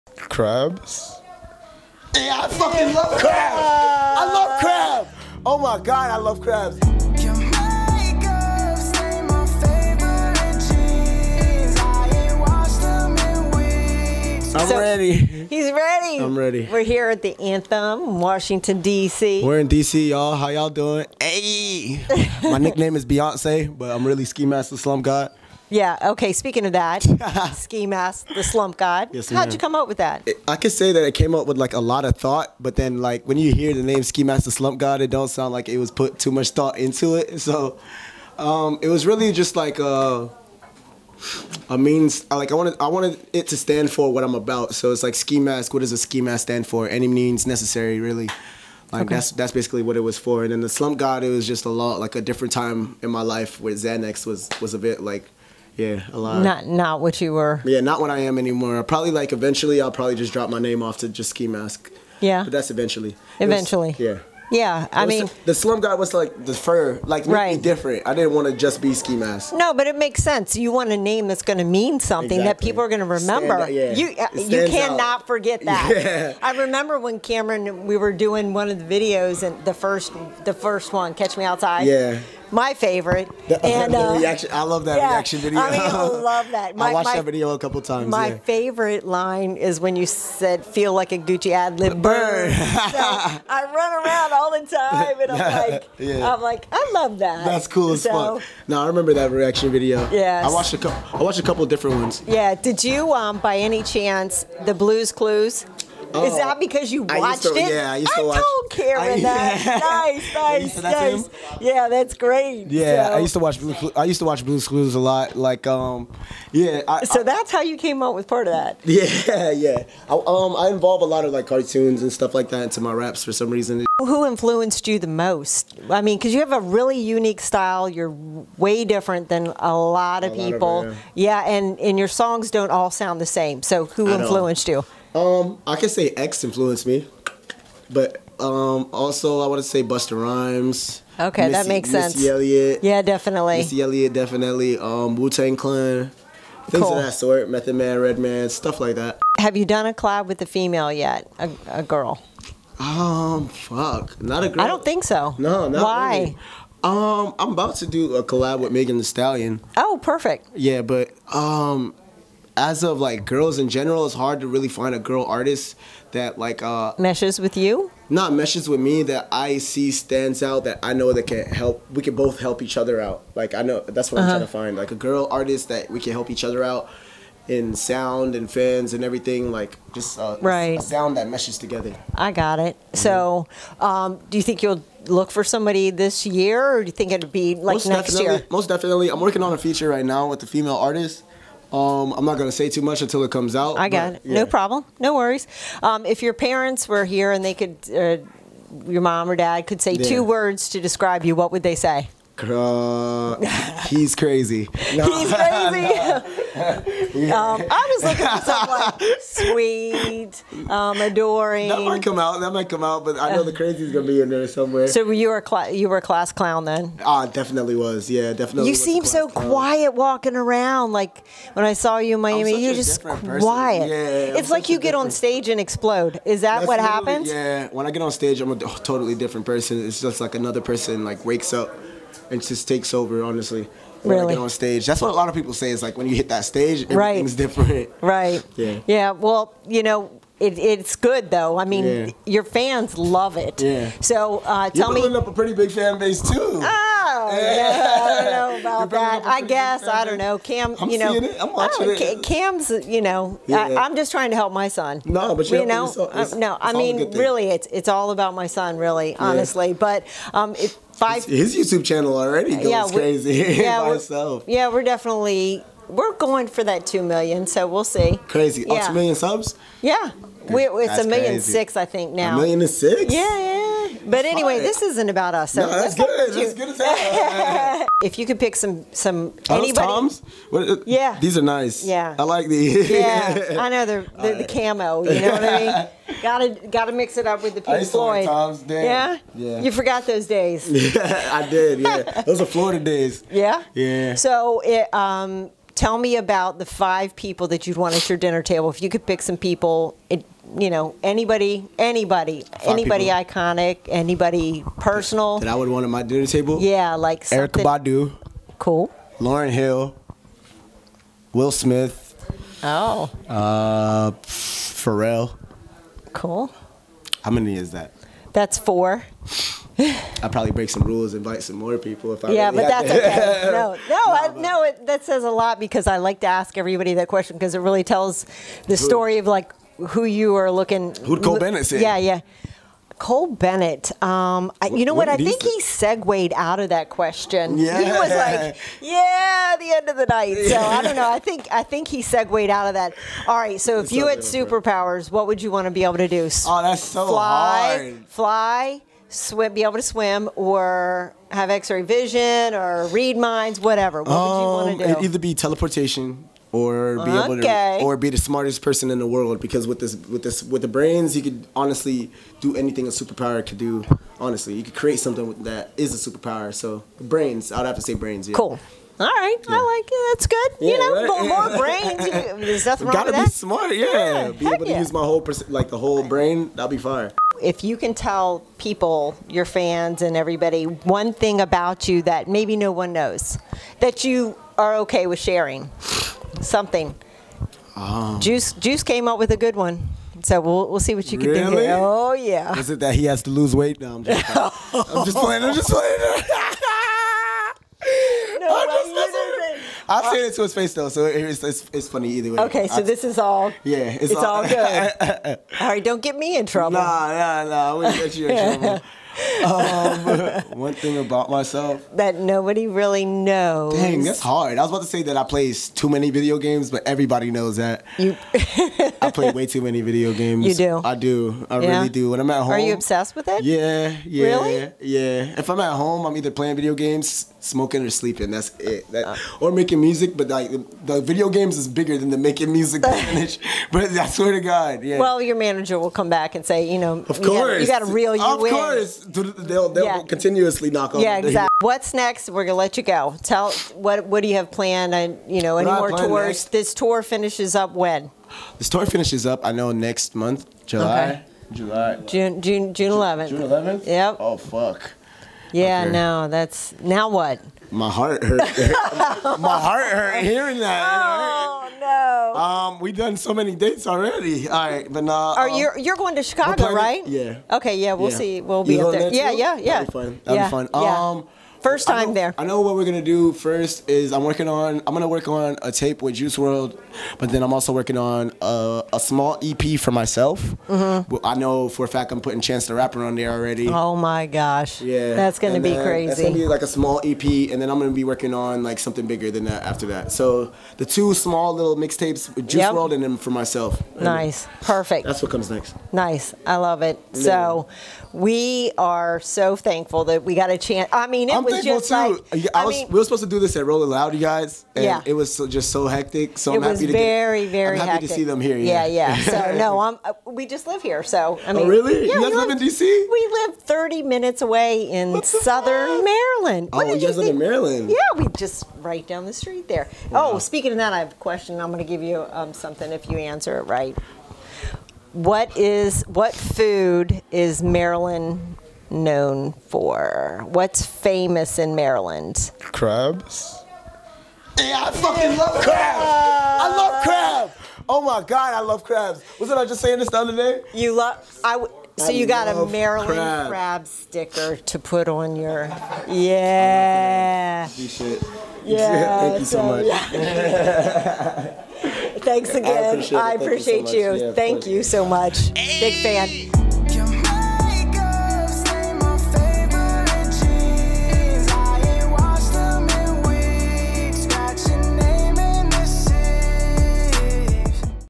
Mom Interviews Ski Mask The Slump God
I got My mom to interview Ski Mask The Slump God! Enjoy.

**EMAIL FOR ALL BIZ INQUIRIES**